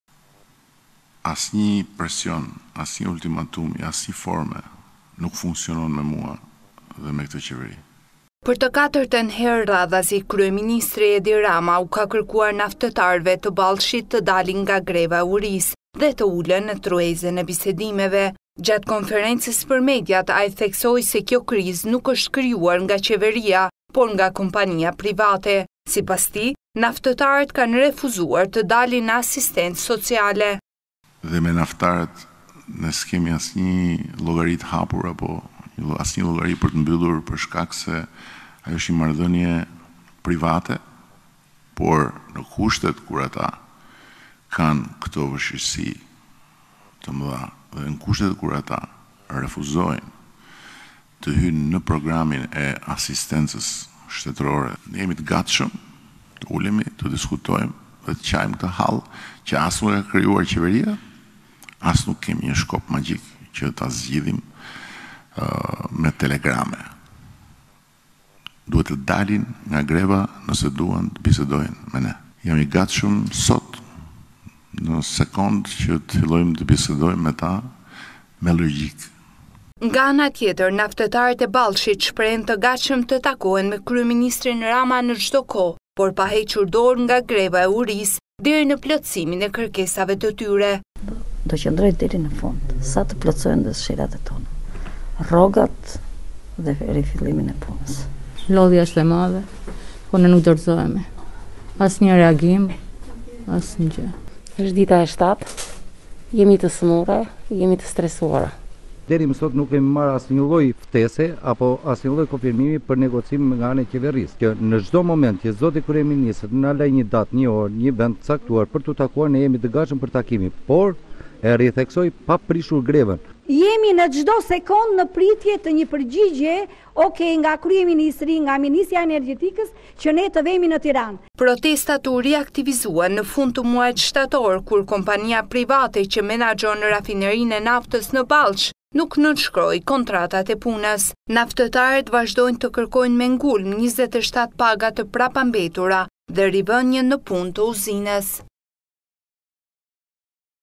Naftëtarët: Cakto datën dhe orën! Një përfaqësi do vijë në Tiranë, por grevën s'e ndalim!
TIRANË- Në ditën e 7 të grevës, palët nuk heqin dorë nga kërkesat e tyre. Kryeministri Edi Rama u ka bërë për të katërtën herë thirrje që të dalin nga greva në mënyrë që të nisin bisedimet për zgjedhjen e një zgjidhjeje të këtij problemi që vijon prej vitesh. Dalja nga greva është kusht, në të kundërtën, kreu i qeverisë e ka ripërsëritur në mesazhin e tij të fundit, se nuk ka bisedime.